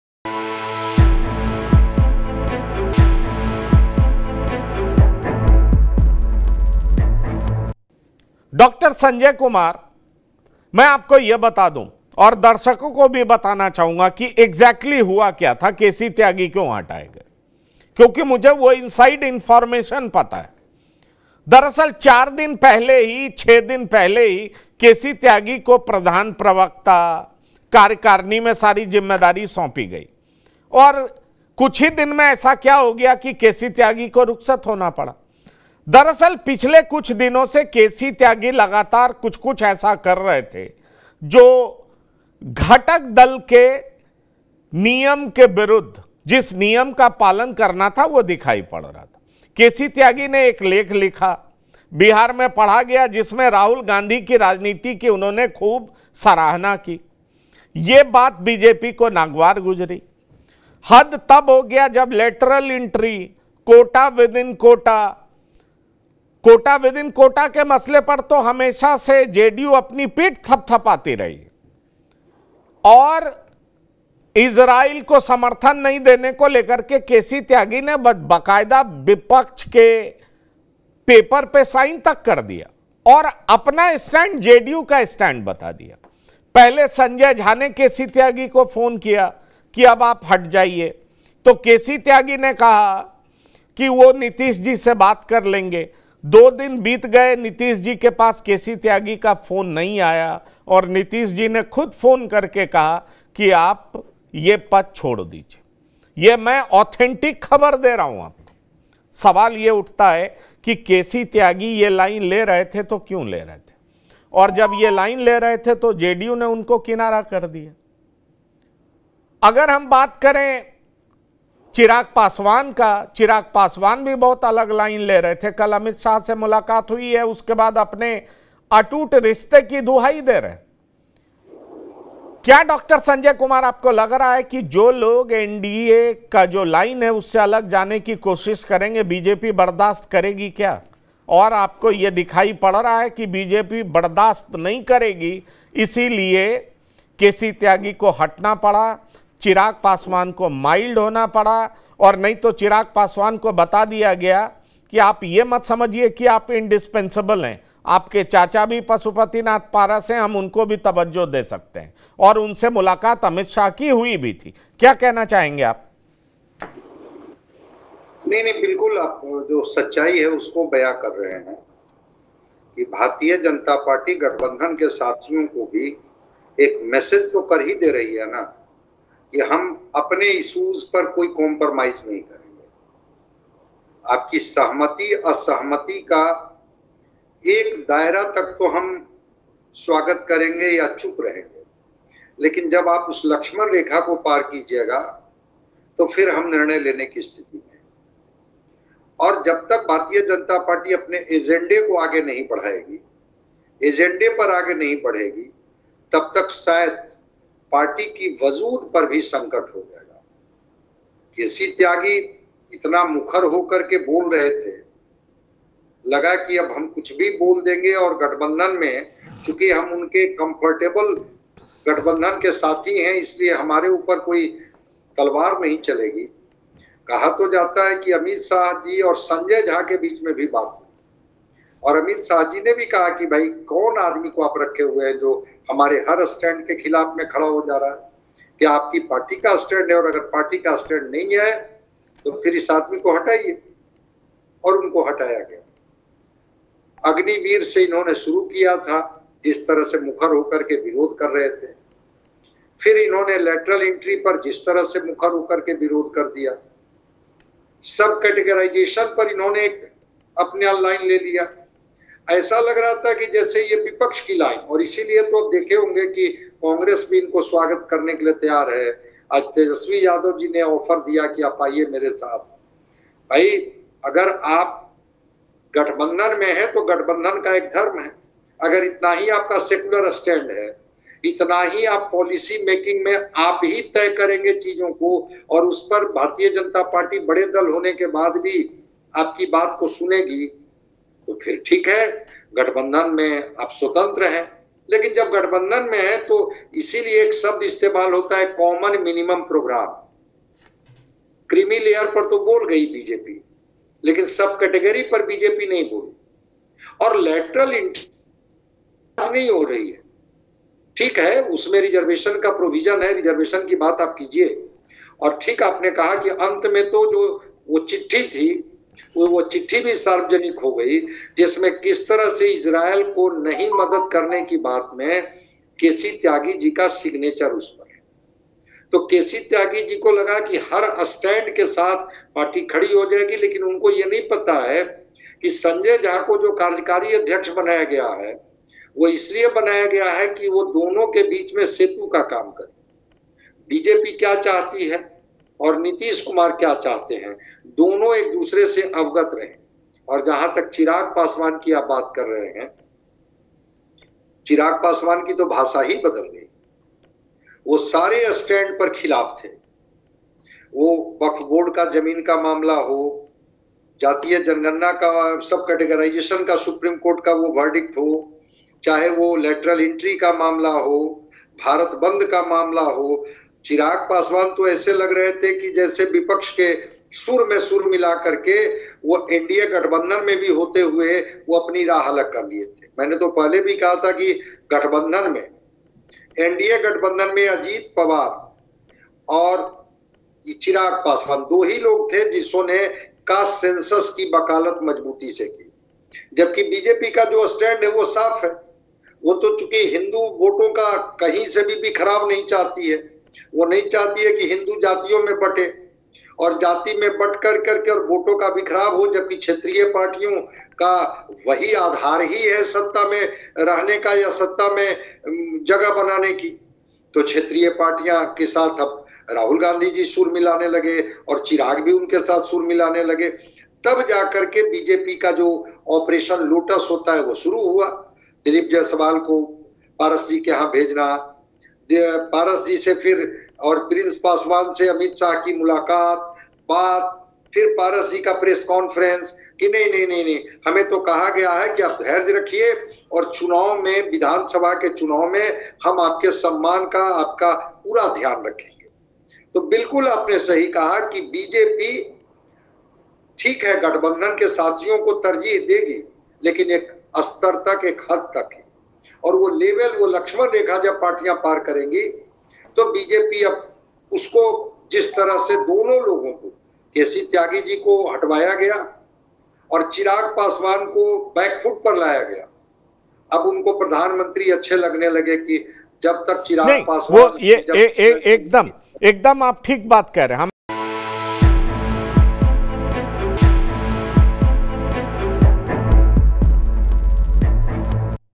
Chirag Paswan अचानक से कैसे बदले? KC Tyagi की विदाई की असल वजह क्या? | Bihar News
हमसे जुड़िए.